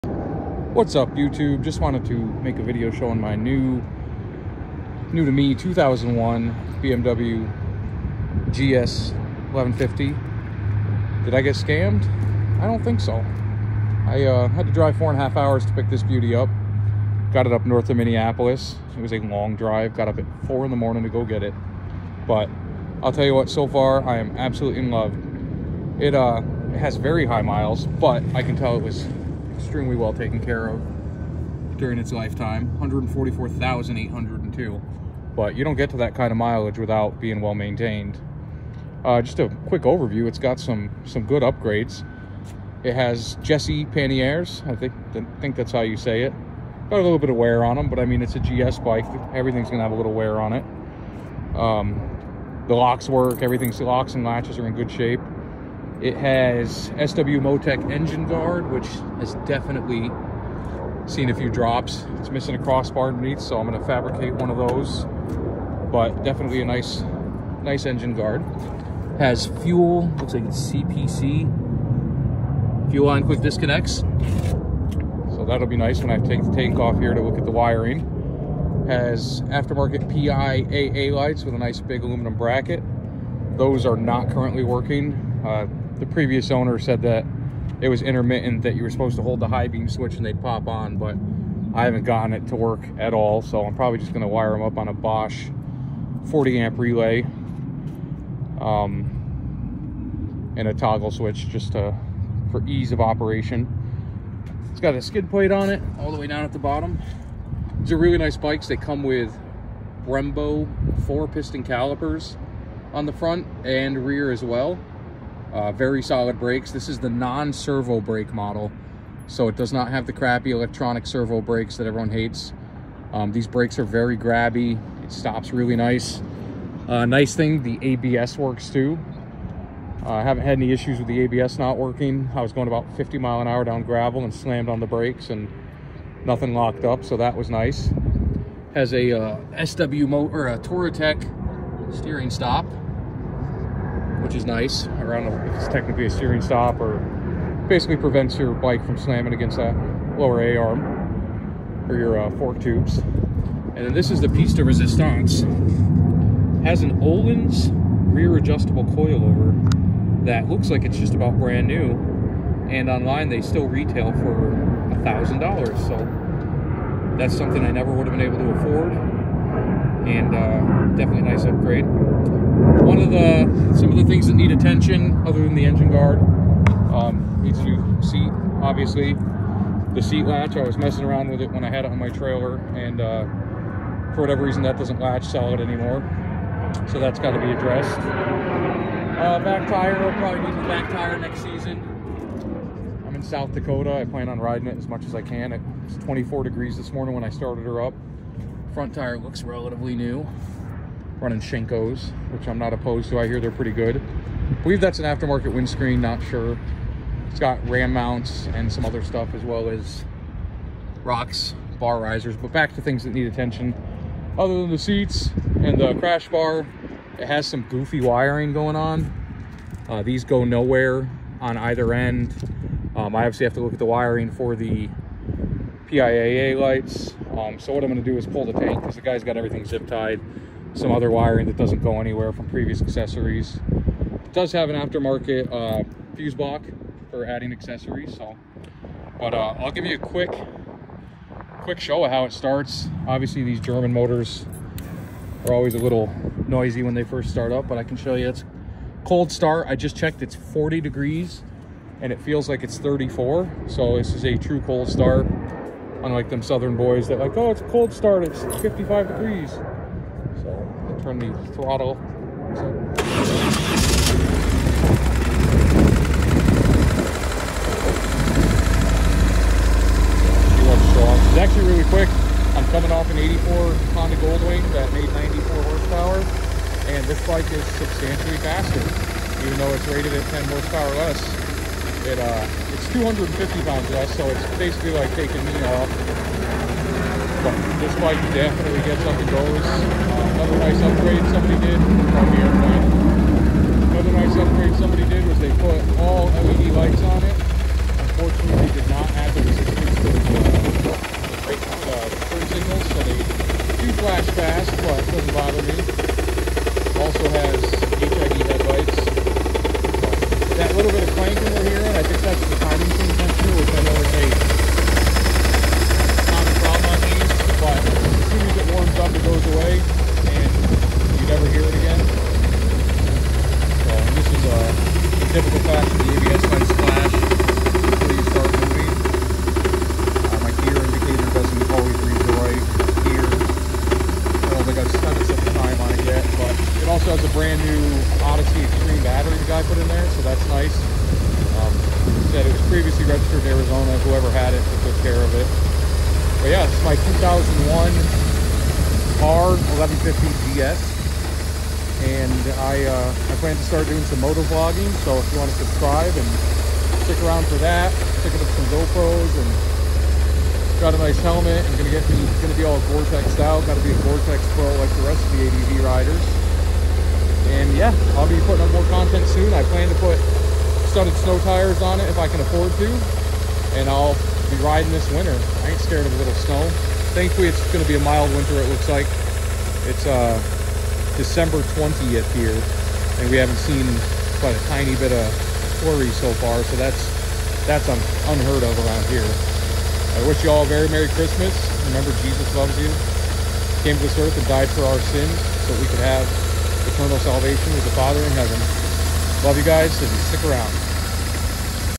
Buying a High Milage R1150GS
Just picked up my first GS bike. Want to start motovlogging but did I get the right bike?